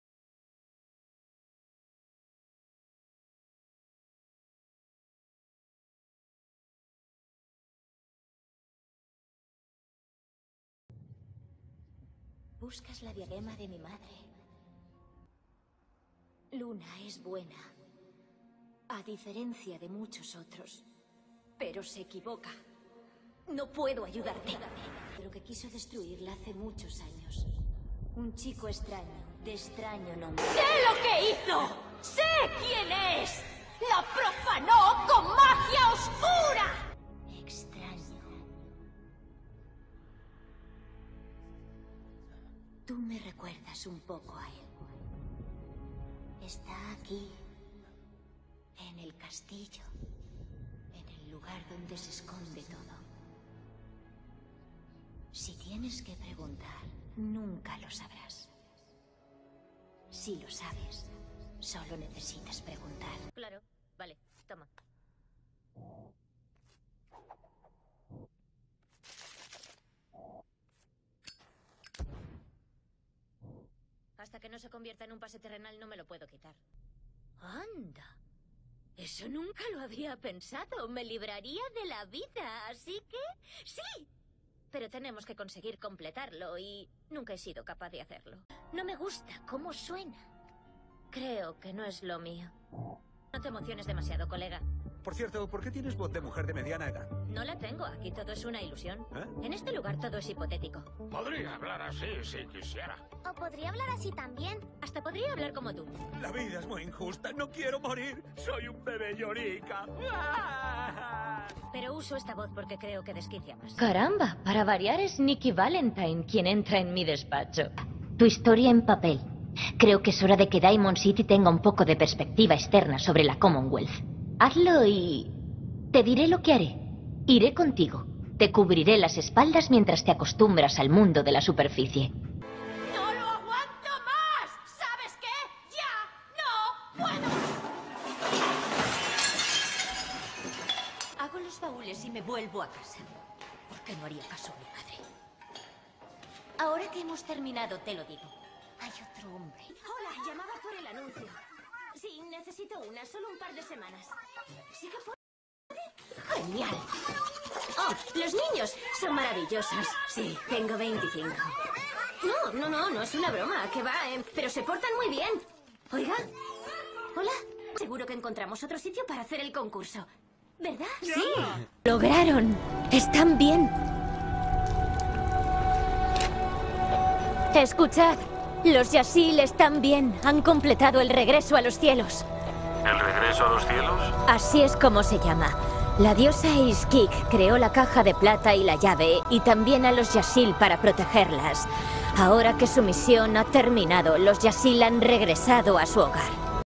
Muestra de Doblaje de Danai Jiménez Querol | Actriz de Doblaje de España
Actriz de Doblaje Española de padre colombiano(por eso es tan linda).
Doblaje de España con un toque latino